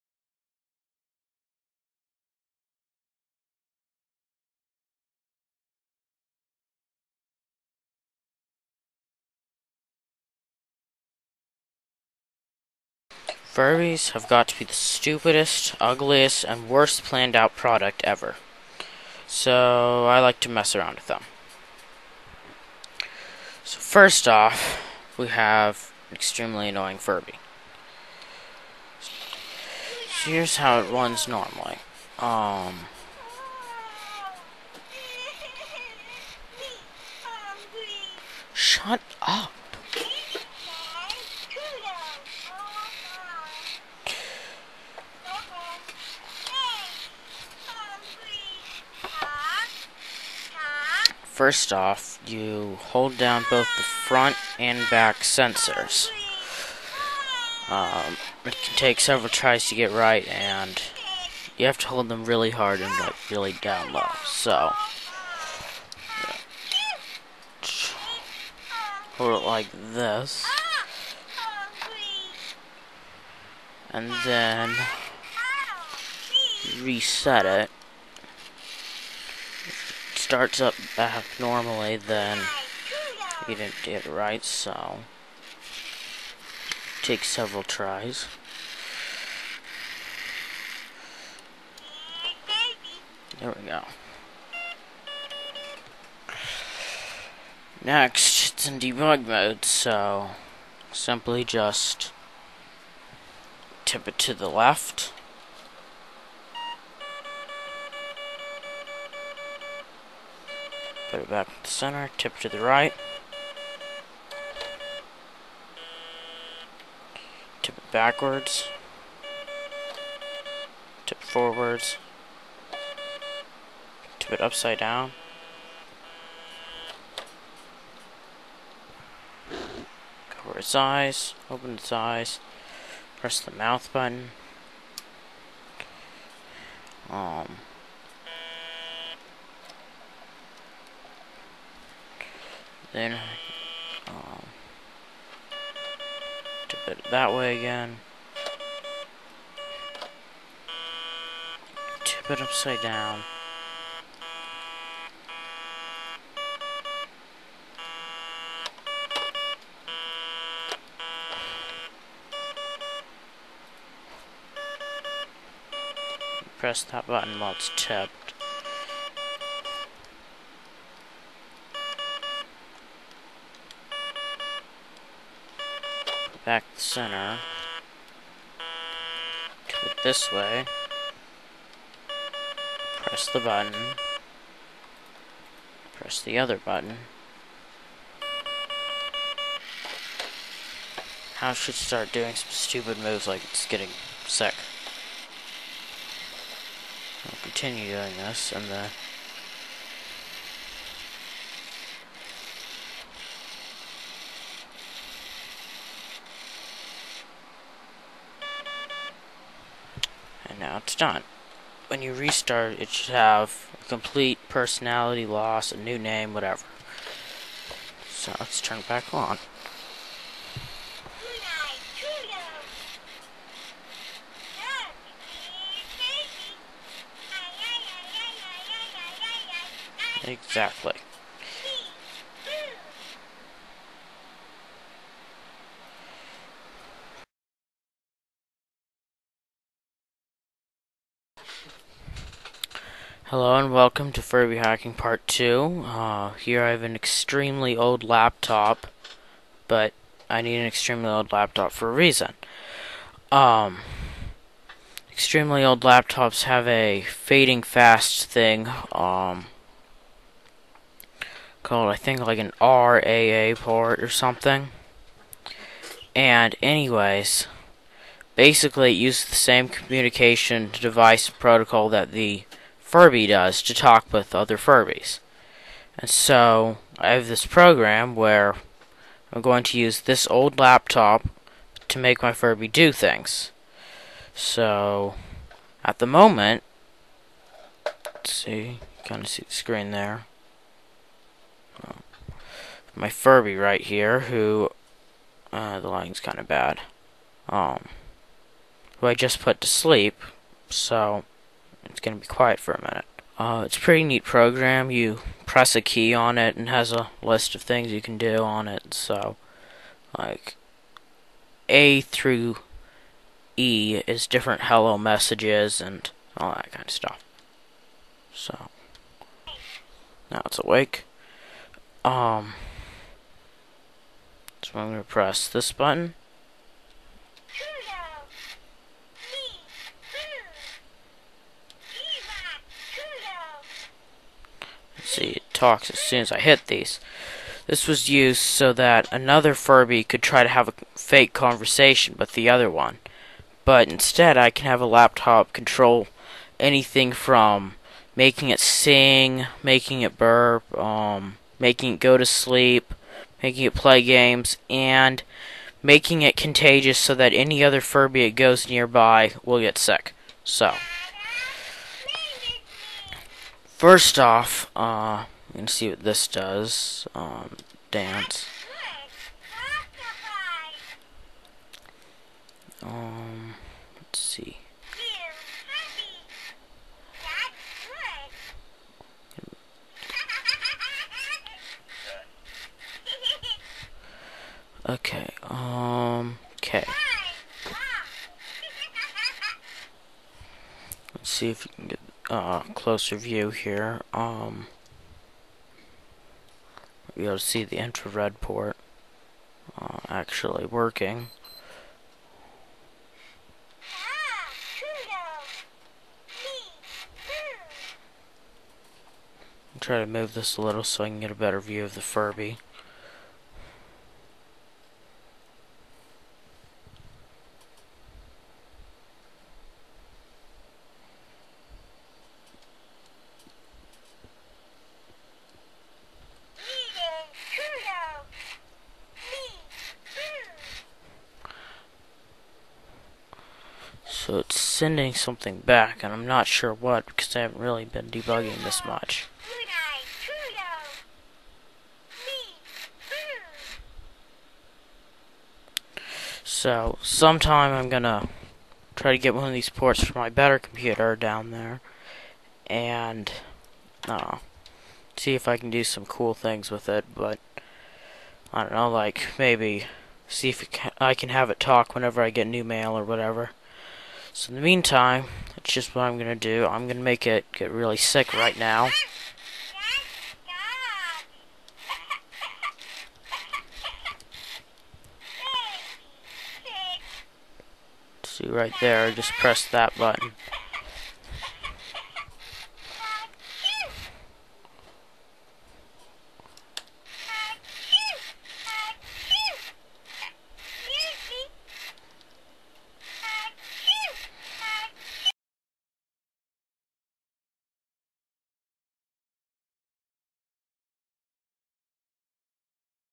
Furby Hacking and Debug Mode - Society Of Code
You can put your Furby in debug mode with these steps:

o Hold down tickle sensor
o Hold down pat sensor
o Reset

It may take several times to get right.

Thanks, Veg!